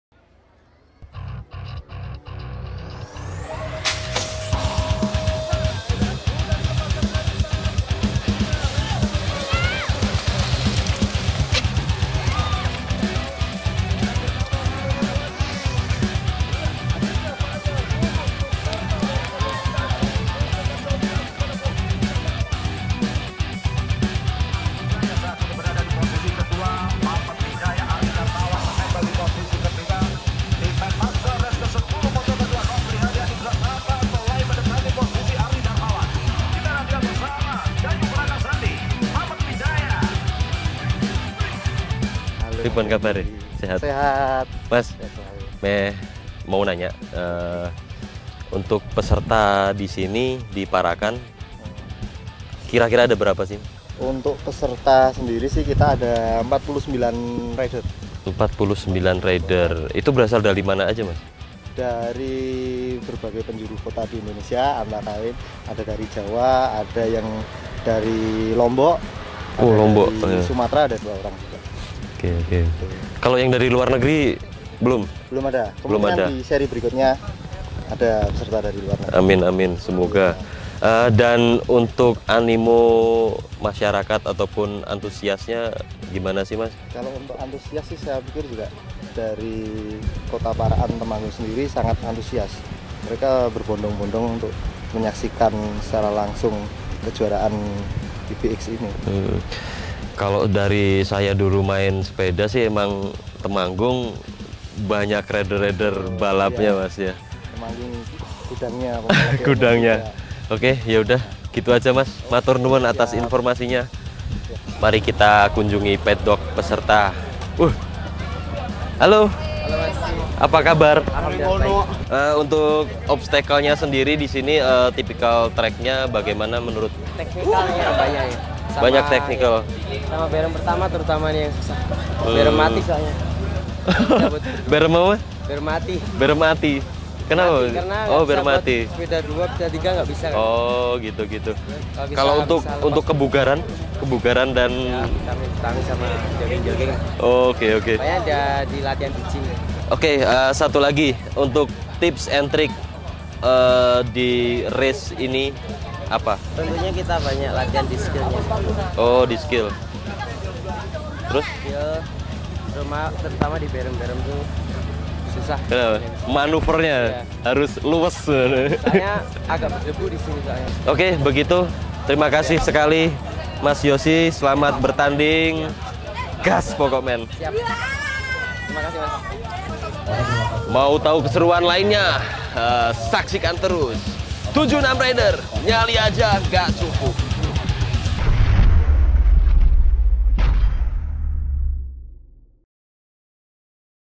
Rider Vlog: Adventure Wendy in 76 Indonesian BMX
Keseruan Wendy kunjungi event 76 Indonesia BMX seri 1 di Parakan, Temanggung. Wendy kunjungi paddock rider, berbincang hangat dengan penyelenggara dan saksikan sengitnya komperisi. Simak lengkap hanya di Channel Youtube 76Rider!